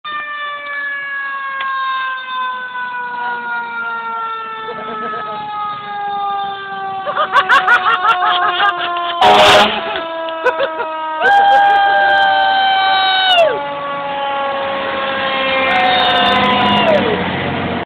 221 gets a run (The night of a full moon!)
Caledonia's Enging 221 gets a run, and you never know what you will see on a night of a full moon.